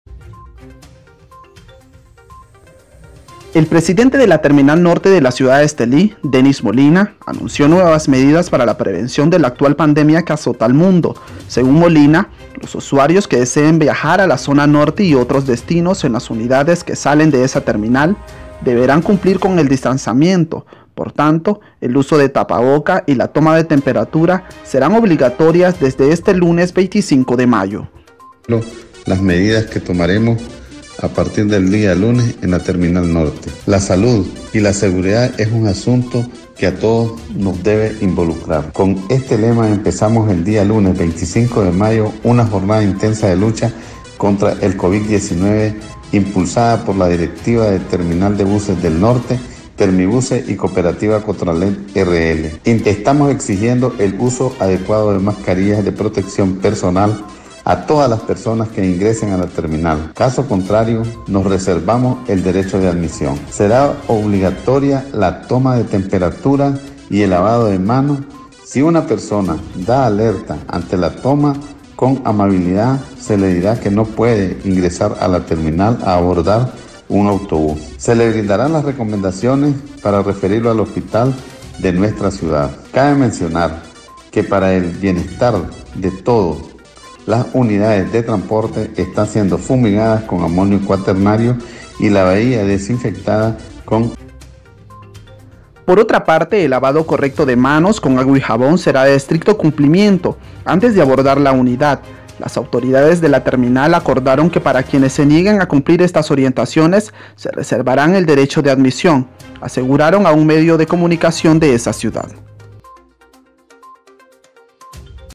Terminal de buses de Estelí exigirá mascarillas, quien no acate orientación no entrará a la Cotran
El presidente de la Terminal Norte de la ciudad de Estelí, Dennis Molina anuncio nuevas medidas para la prevención de la actual pandemia que azota al mundo según Molina los usuarios que deseen viajar a la zona norte y otros destinos en las unidades que salen de esa terminal deberán cumplir con el distanciamiento, por tanto, el uso de tapabocas y la toma de temperatura serán obligatorias desde este lunes 25 de mayo.